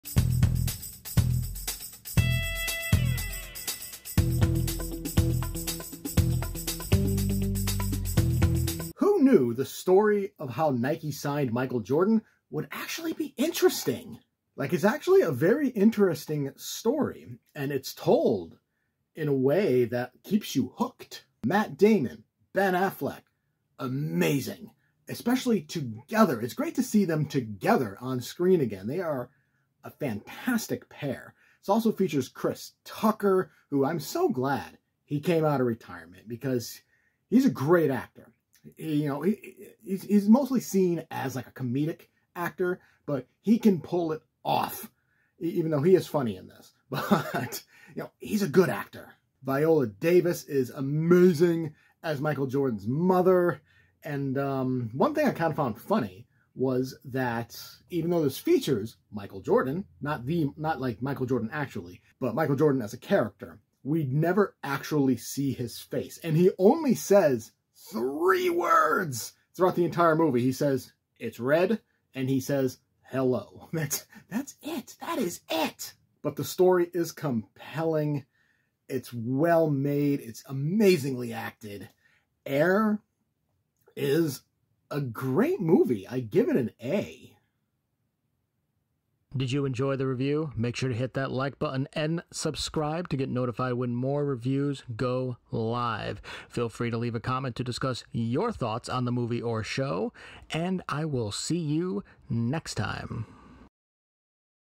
Minute Review Quick Thoughts: Air is the Shoe Story I Never Knew I Needed
Welcome to Minute Review Quick Thoughts where the review is 2 minutes or less! In this episode: AirMovie starring MattDamon (and a LOT more!)